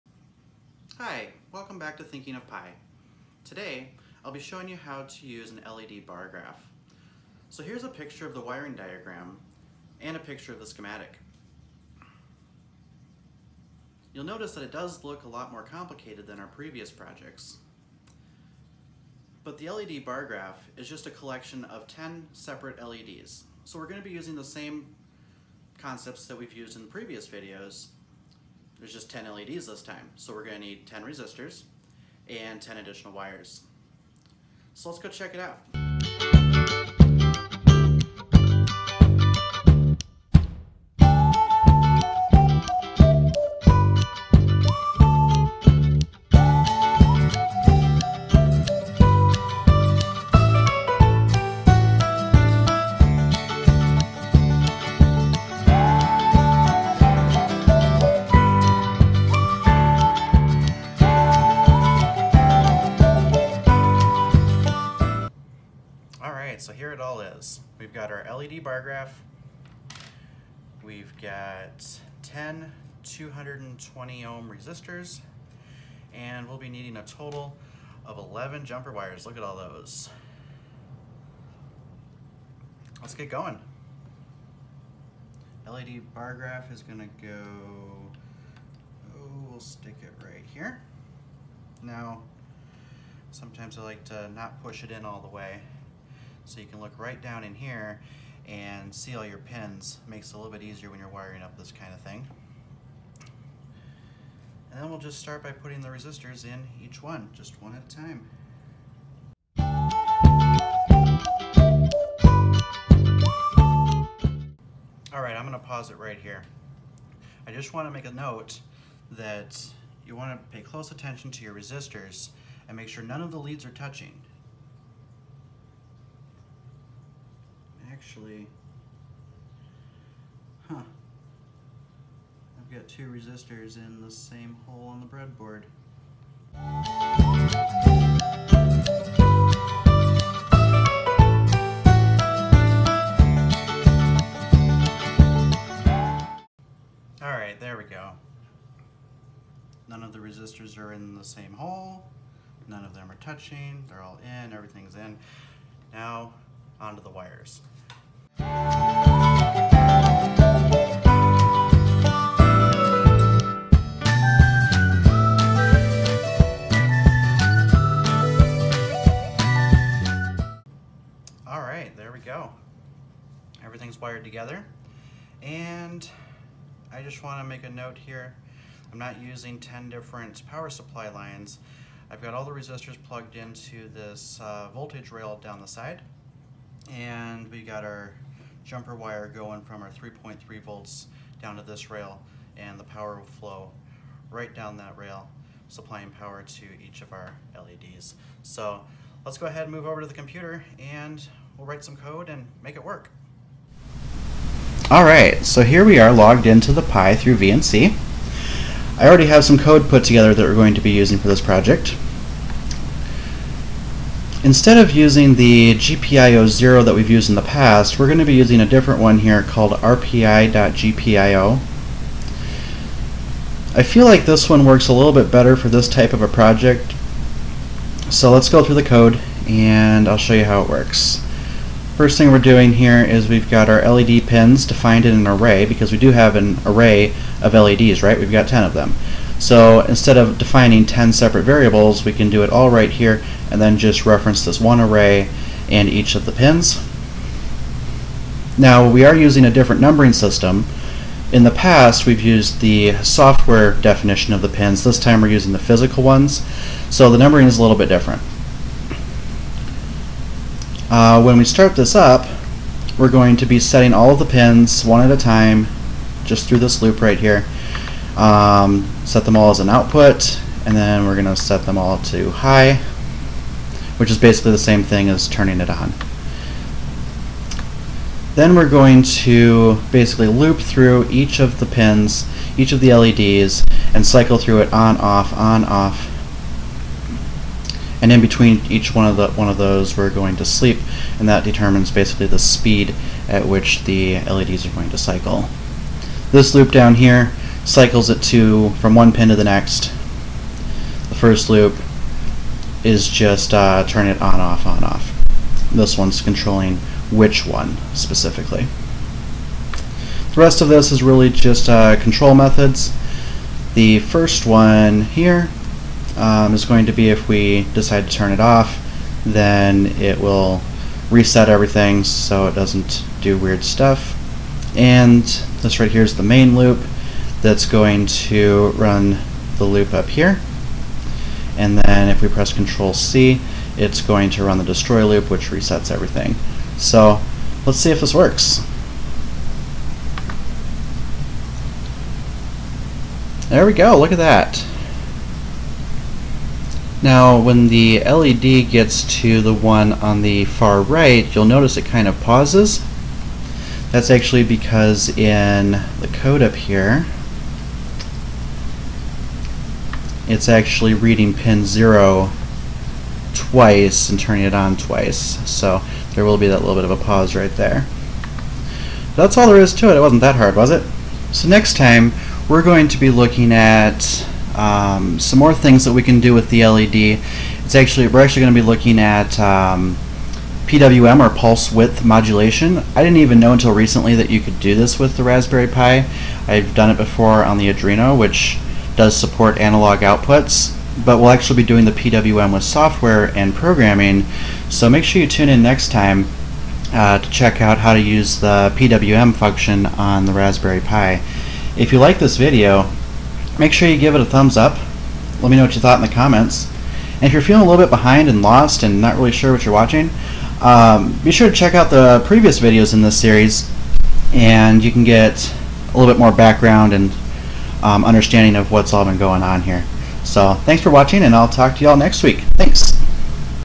LED Bar Graph Array | Raspberry Pi 3B+ | Projects for beginners
In this video we hook up an LED bar graph and cycle through the lights using an array and for loops in Python.j

for the wonderful music!